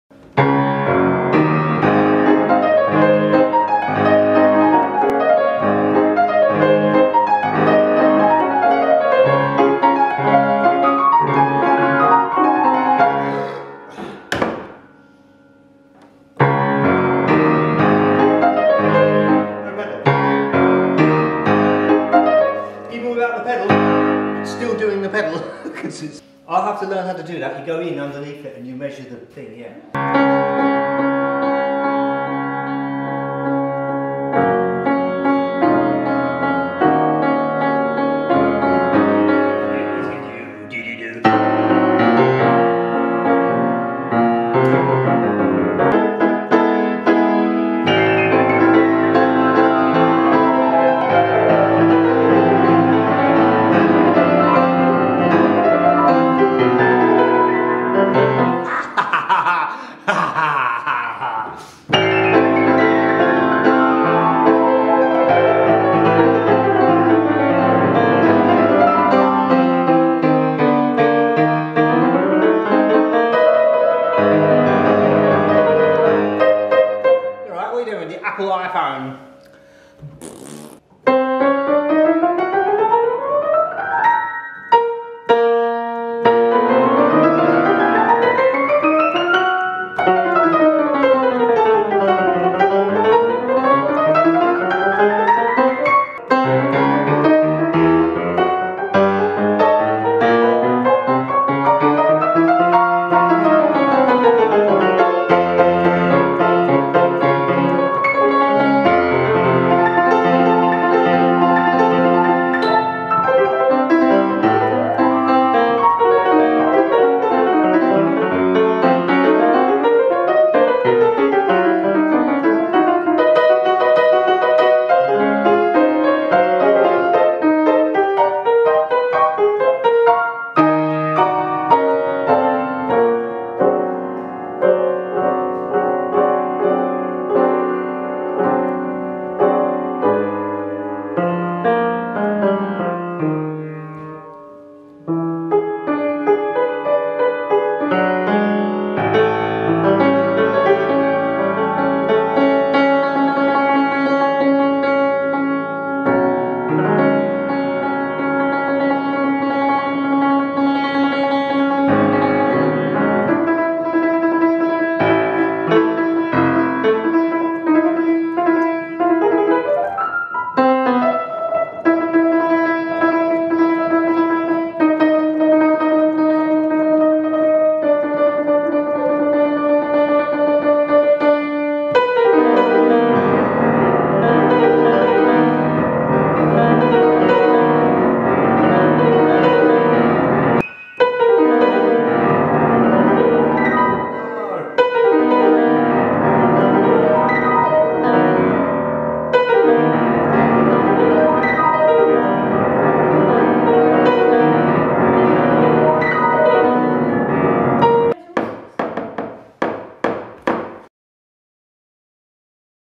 Messing about on Bechstein
1899 bechstein that's a tad brash soundin'! gotta soften up the hammers a bit on the mid to low end etc!